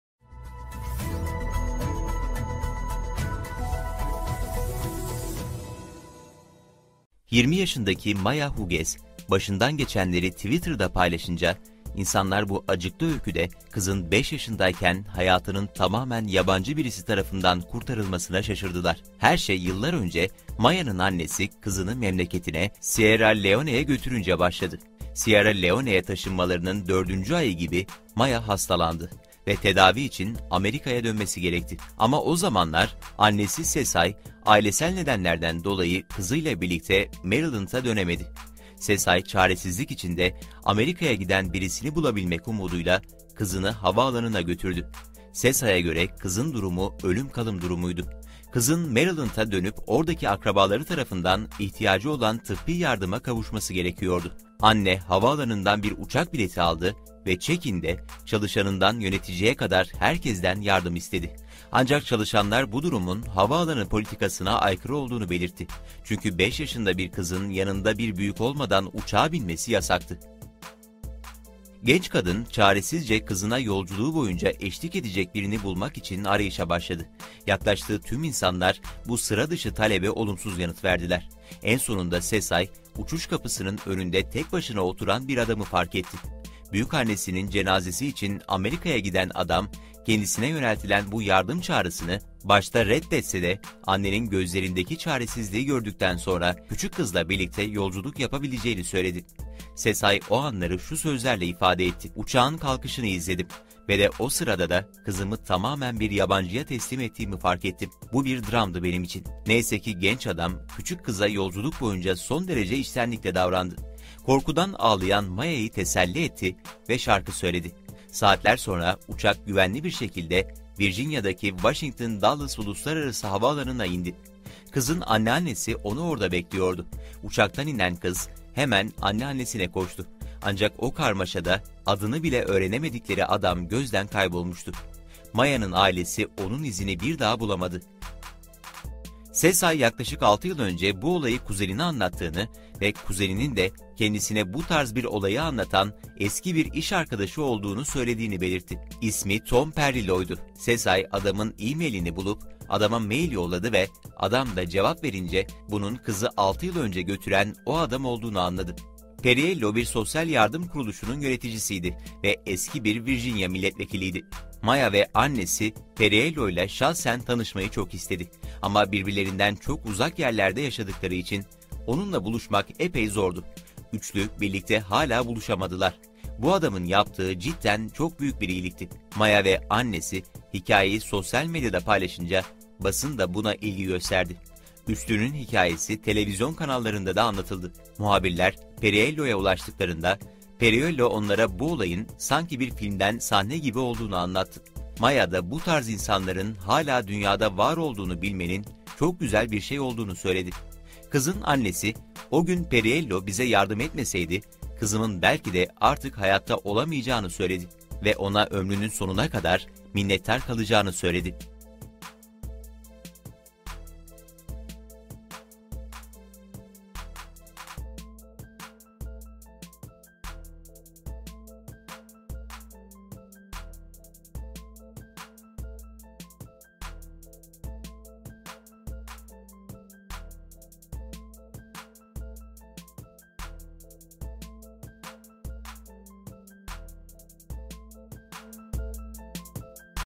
5 Yaşındaki Kız Hiç Tanımadığı Bir Adamla Uçağa Bindi, 15 Yıl Sonra İnanılmaz Bir Şey Oldu
5 Yaşındaki Kız Hiç Tanımadığı Bir Adamla Uçağa Bindi, 15 Yıl Sonra İnanılmaz Bir Şey Oldu

20 yaşındaki Maya Hughes başından geçenleri Twitter'da paylaşında insanlar bu acıklı öyküde kızın 5 yaşındayken hayatının tamamen yabancı birisi tarafından kurtarılmasına şaşırdılar. Her
şey yıllar önce Maya'nın annesi kızını memleketine, Sierra Leone'a götürünce başladı. Sierra Leone'a taşınmalarının 4. ayı gibi Maya hastalandı ve tedavi için Amerika'ya dönmesi gerekti ama o zamanlar annesi sesay ailesel nedenlerden dolayı kızıyla birlikte Maryland'a dönemedi. Sesay çaresizlik içinde, Amerika'ya giden birisini bulabilmek umuduyla kızını havaalanına götürdü. Sesay'a göre kızın durumu ölüm kalım durumuydu. Kızın Maryland'a dönüp oradaki akrabaları tarafından ihtiyacı olan tıbbi yardıma kavuşması gerekiyordu. Anne havaalanından bir uçak bileti aldı ve check-in'de çalışanından yöneticiye kadar herkesten yardım istedi. Ancak çalışanlar bu durumun havaalanı politikasına aykırı olduğunu belirtti. Çünkü 5 yaşında bir kızın yanında bir büyük olmadan uçağa binmesi yasaktı.